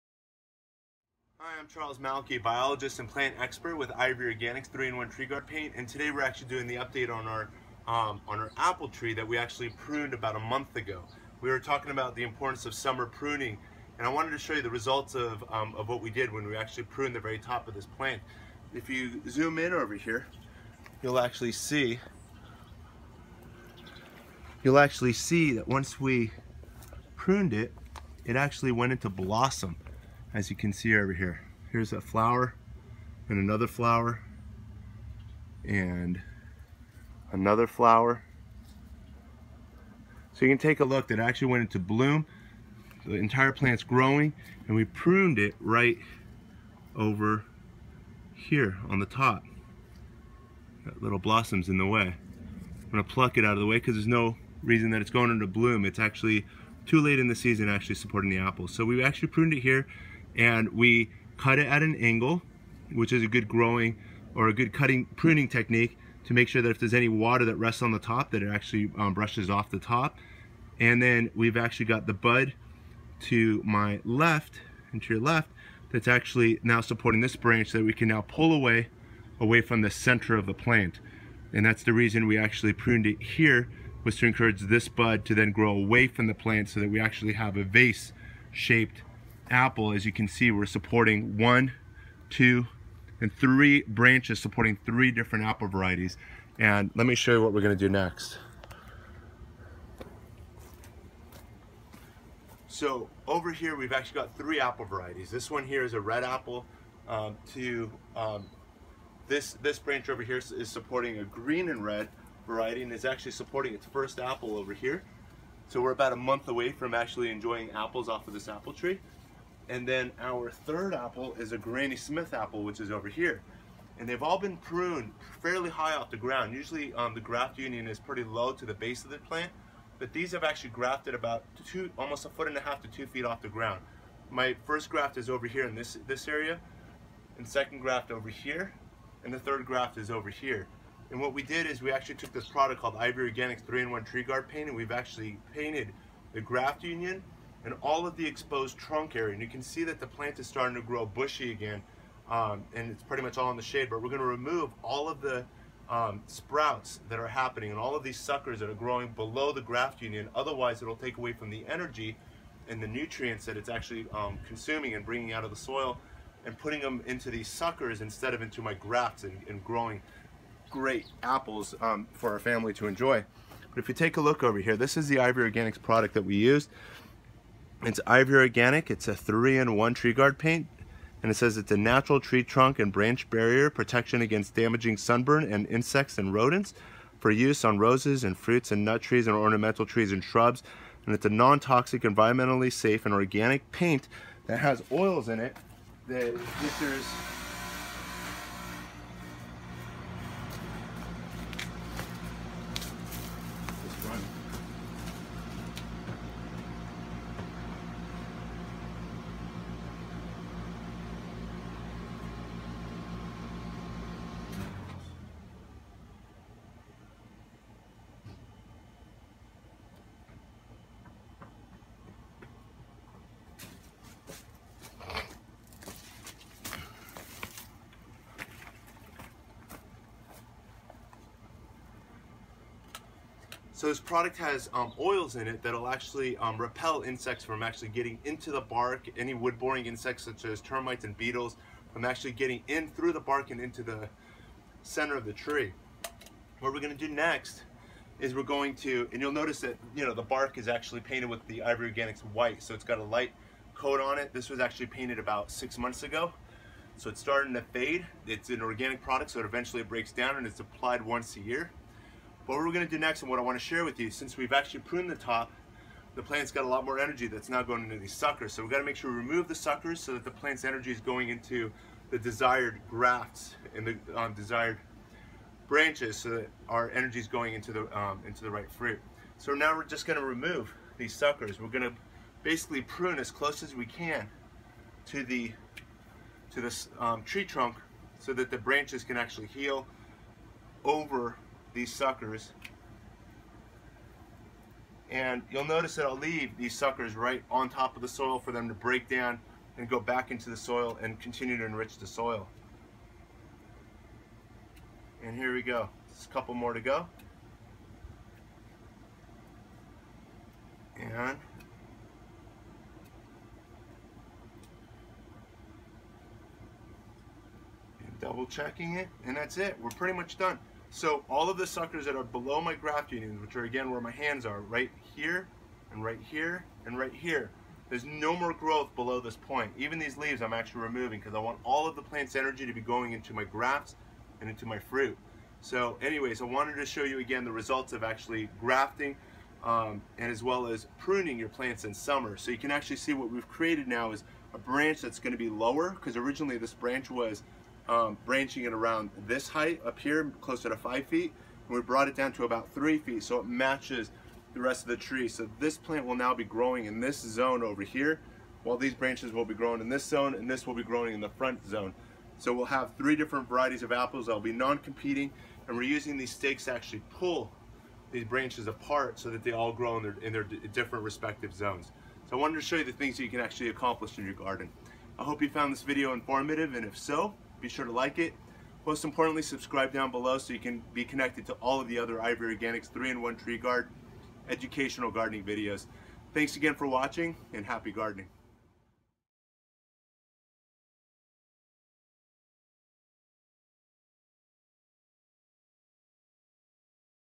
Summer-Time Pruning Of Fruit Trees For More Spring Flowers & Fruits! |30-DAYS FOLLOW-UP
30-days follow up to the benefits of summer pruning for fruit trees to manage the size of your tree, to maximize flower and fruit the following year, and ultimately create a stronger plant to support years of fruit production. In this video, an apple tree was used; however, the principles taught in this video can be applied to most fruit trees.